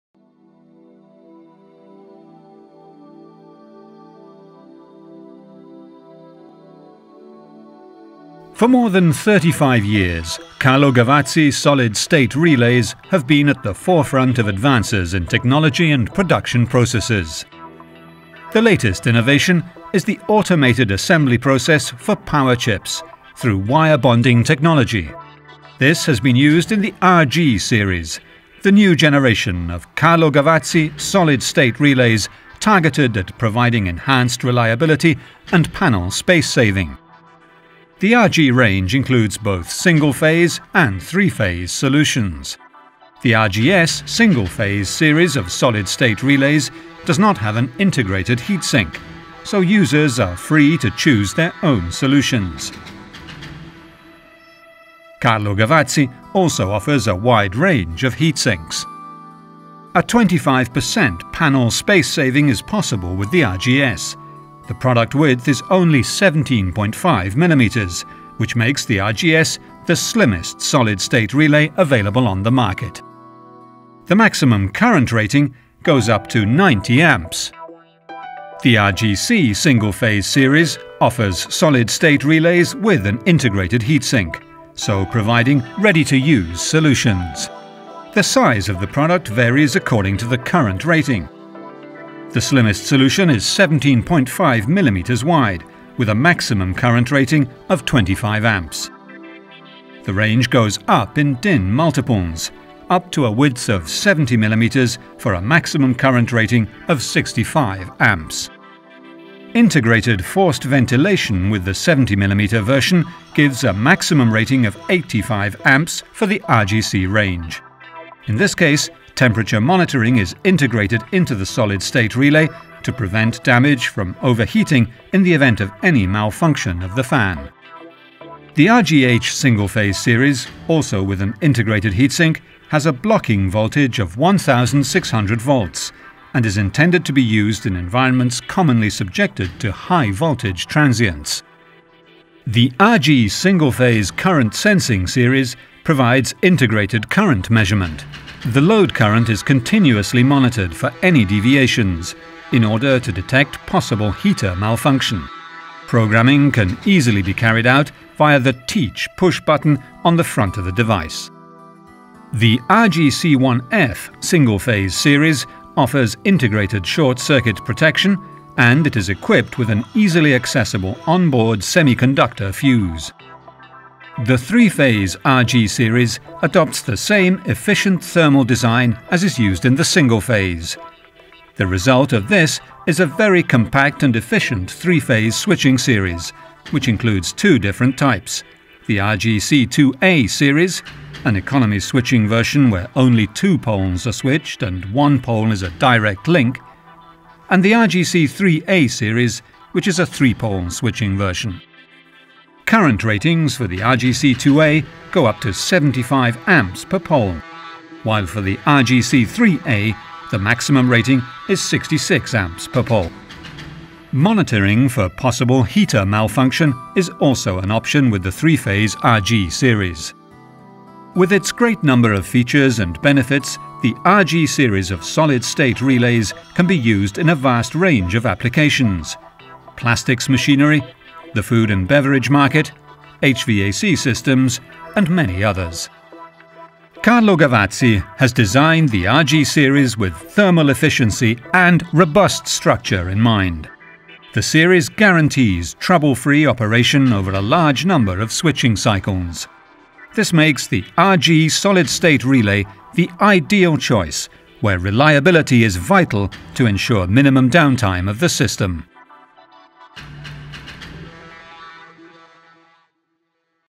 Series RG: Relés y Contactores de estado sólido
La serie RG adopta un innovador diseño en eficiencia térmica que
la convierte en la solución más compacta dentro de nuestra
amplia gama, con un ahorro de espacio en panel del 25%.

Además de sus funciones de conmutación en CA y CC, incluye
variantes sobre mejoras anteriores e integra más funciones que las
típicamente asociadas a relés y contactores estáticos.
La fabricación de la serie RG se realiza en nuestra fábrica que
cumple la norma ISO 9001 y que ha sido también homologada
según ISO 140001.